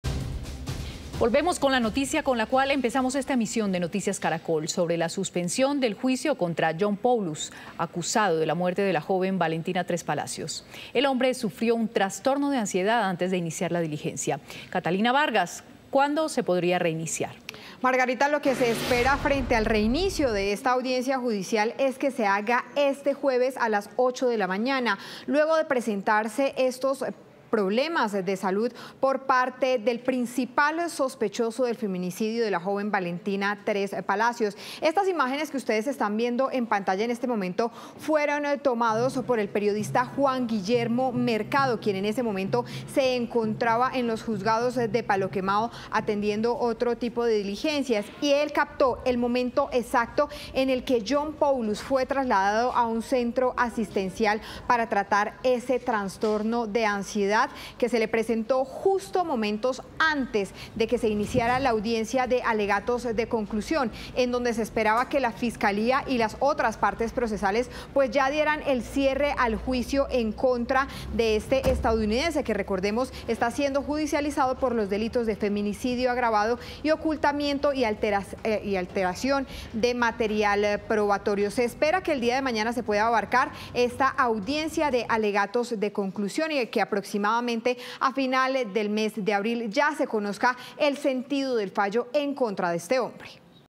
Video del momento en que John Poulos es llevado a un hospital tras sufrir ataque de ansiedad
En la mañana de este miércoles, 17 de abril de 2024, el estadounidense John Poulos, principal sospechoso del feminicidio de la DJ Valentina Trespalacios, sufrió un trastorno de ansiedad y tuvo que ser trasladado a un centro asistencial. La nueva audiencia ya tiene fecha y hora.